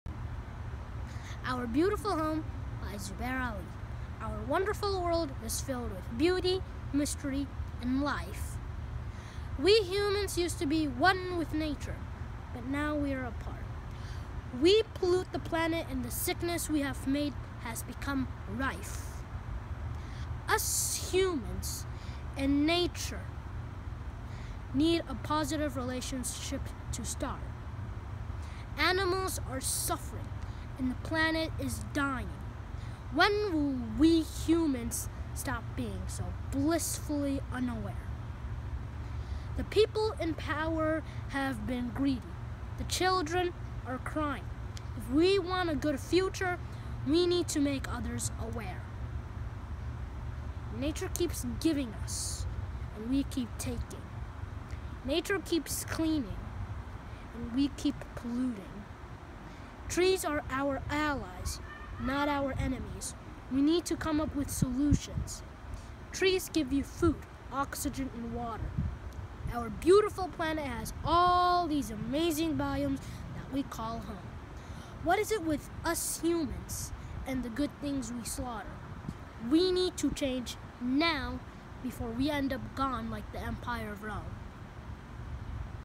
Our Beautiful Home By: Zubair Ali
Eco Poem for Student Environmental Action Showcase 2021